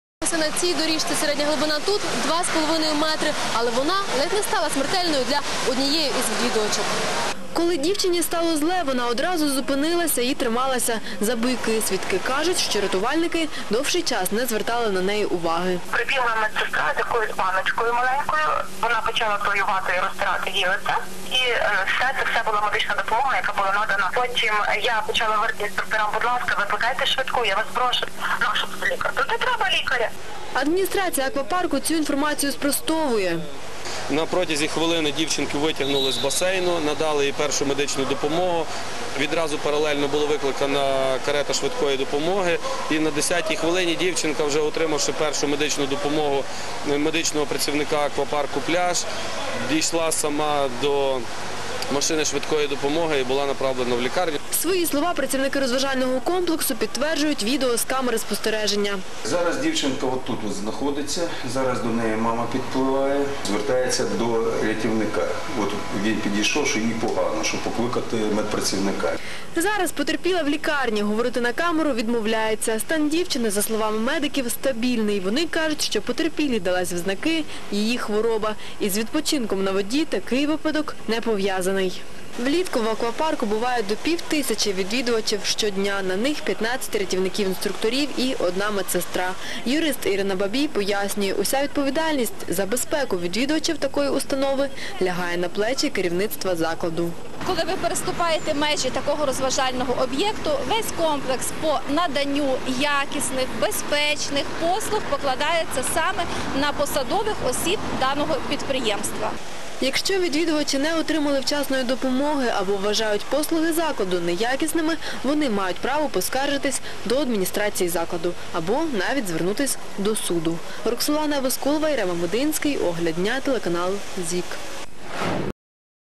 В аквапарку
У львівському аквапарку молодій дівчині стало погано і вона пішла під воду.